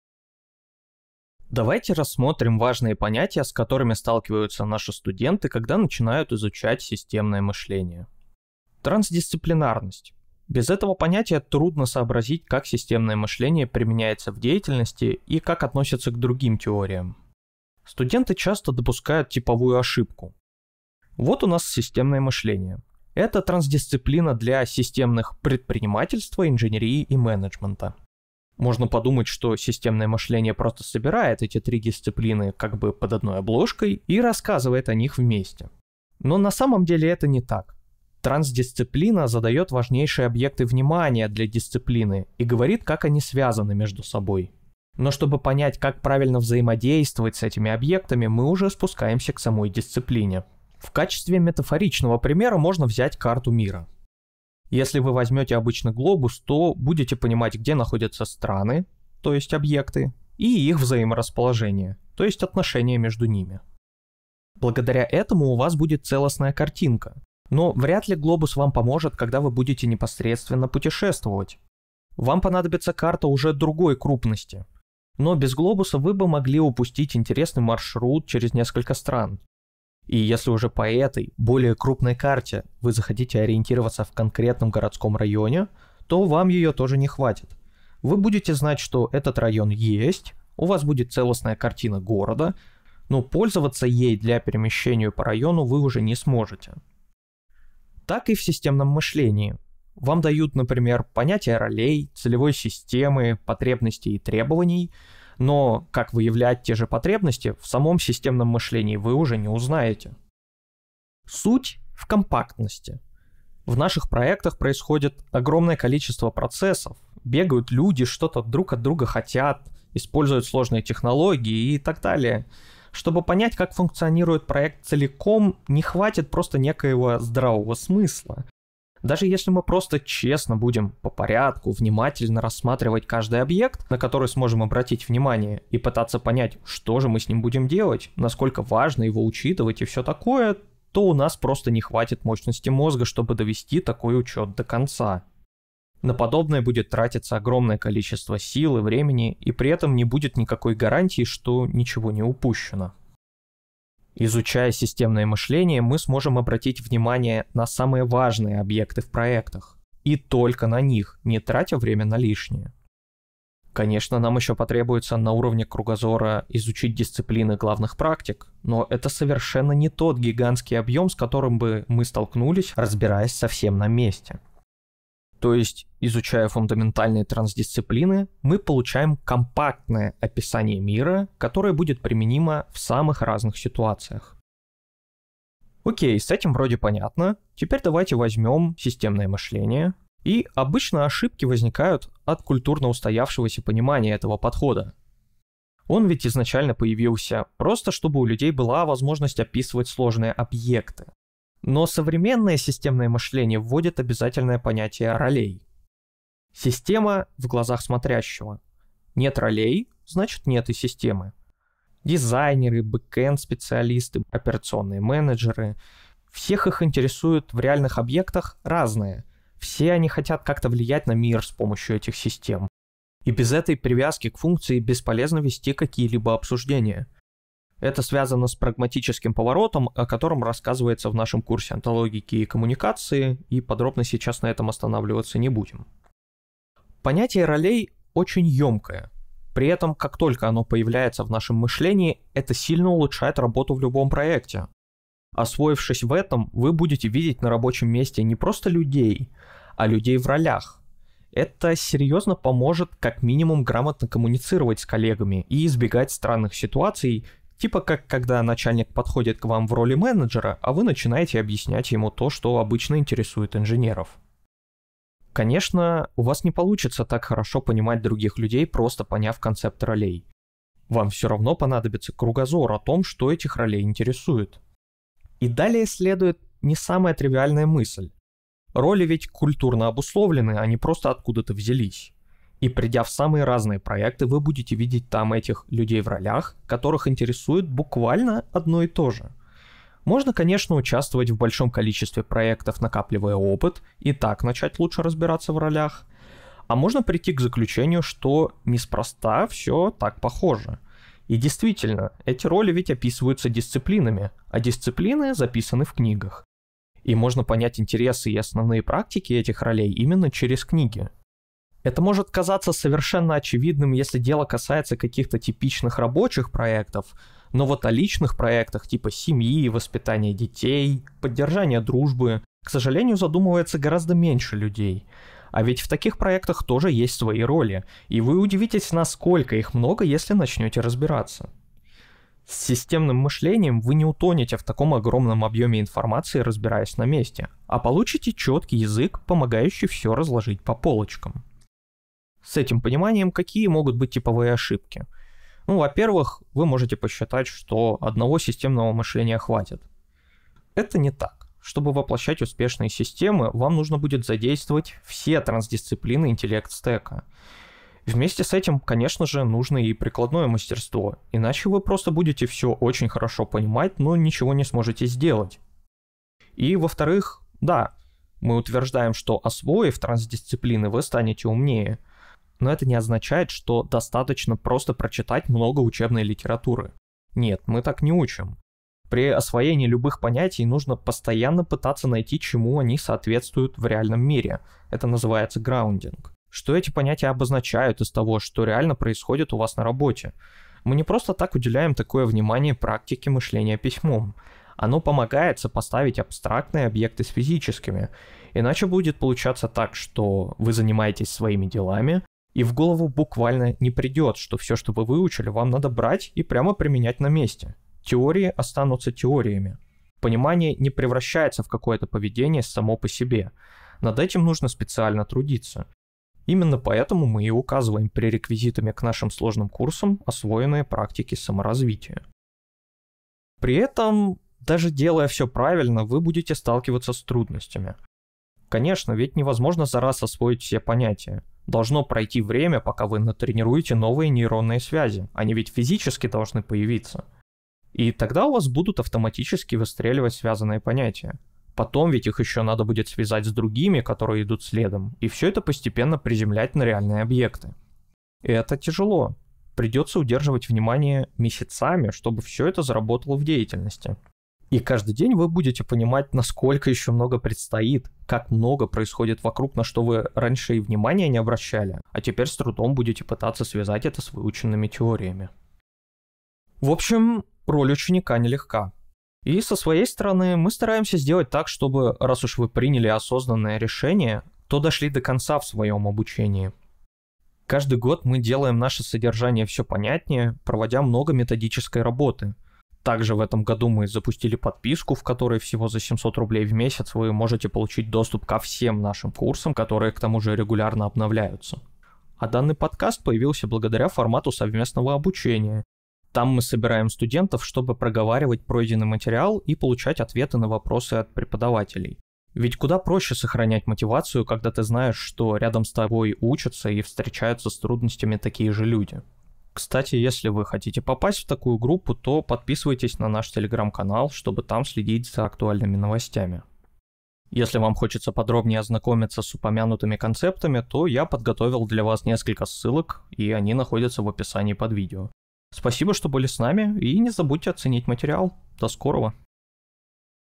Разбираем понятия трансдисциплинарности и ролей
Текстовые материалы:
3. Что такое ролевое мастерство
4. Связанные с "ролями" понятия

1 марта 2022 Прошла первая встреча совместного обучения введению в системное мышление. Это краткая выжимка.